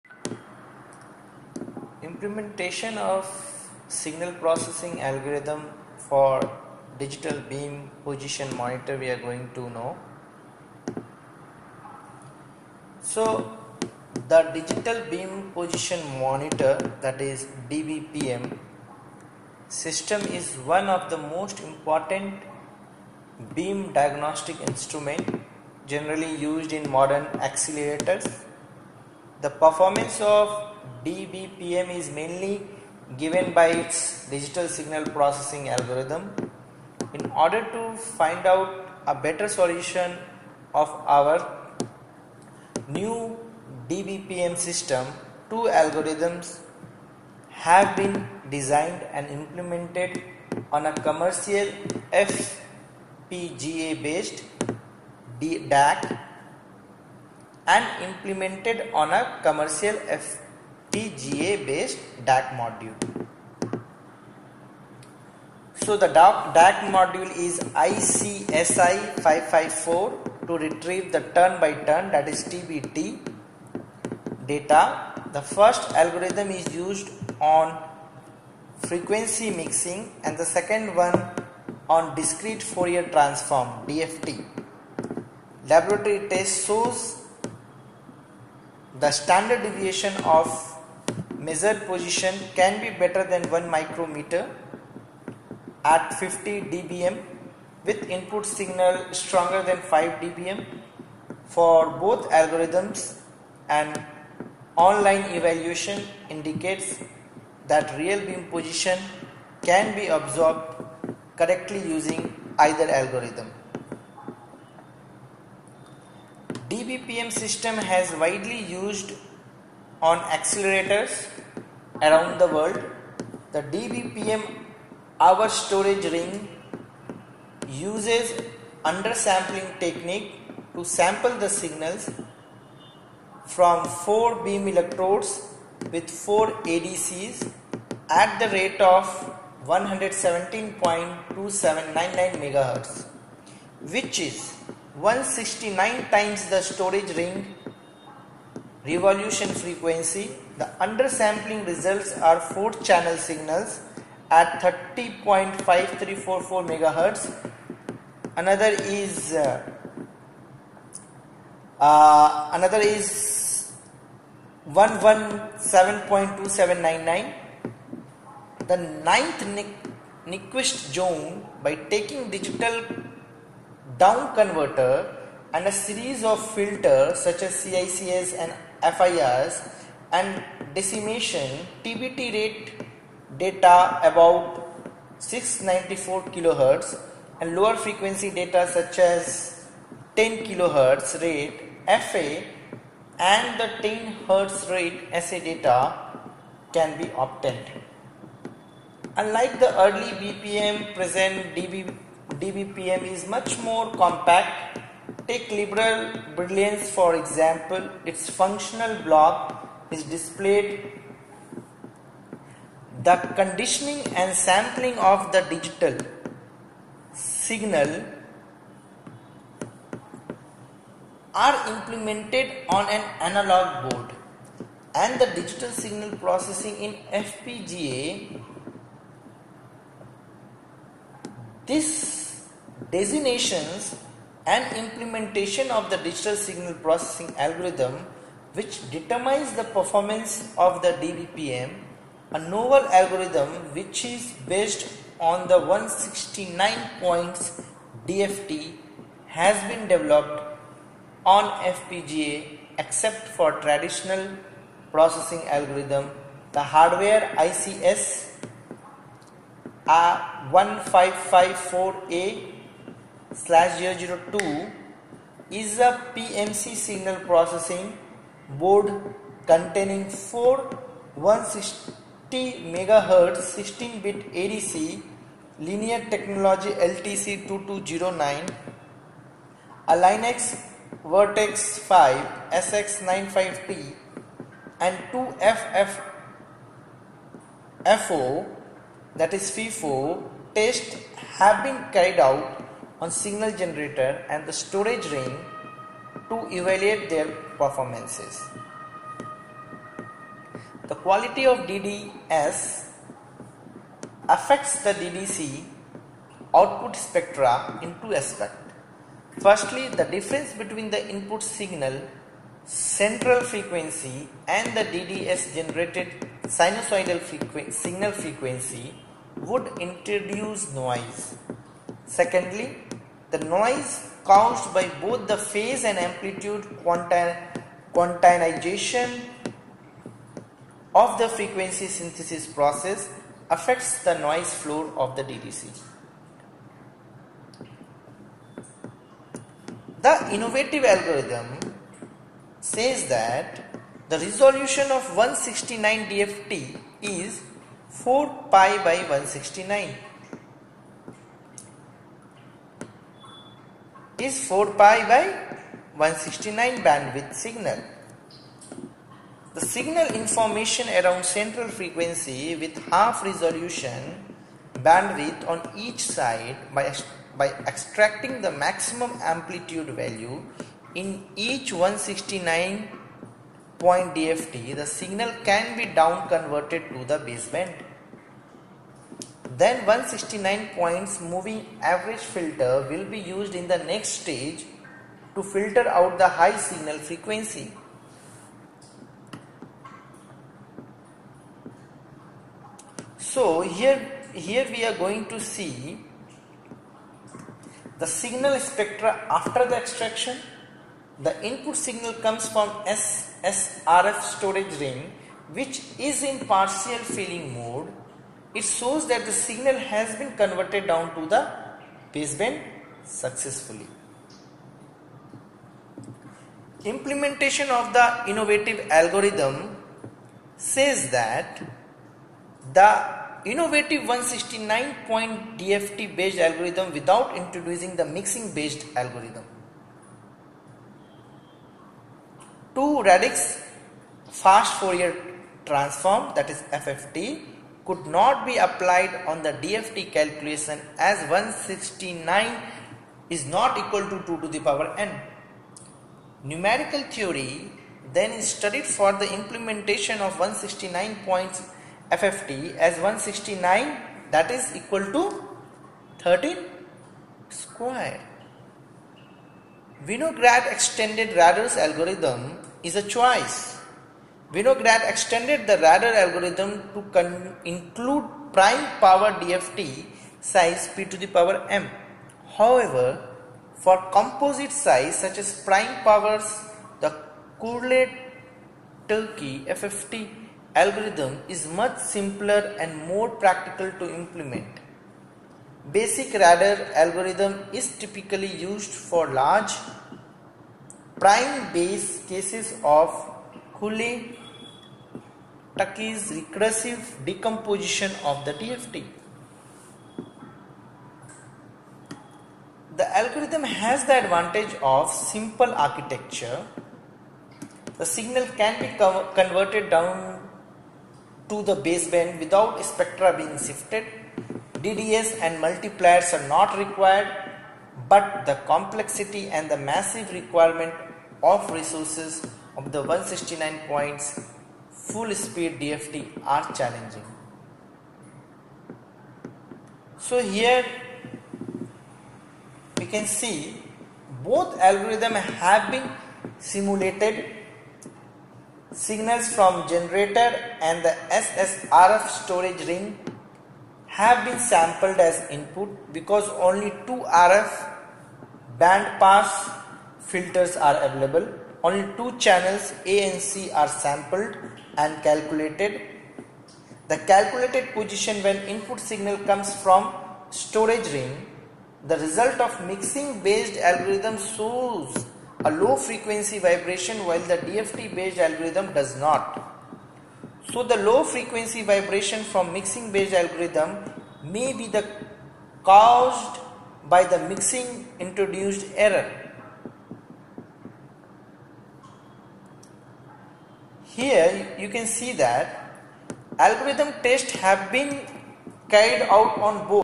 Implementation of Signal Processing Algorithm for Digital Beam Position Monitor
Digital beam position monitor ( DBPM) system is one of the most important beam diagnostic instruments generally used in modern accelerators . The performance of DBPM is mainly given by it's Digital Signal Processing algorithm. In order to find out a better solution for our new DBPM system two algorithm have been designed and implemented on a commercial FPGA based DAQ module ( ICS 1554) to retrieve the turn by turn (TBT ) data. The first algorithm is based on frequency mixing and the second one on discrete Fourier transform (DFT).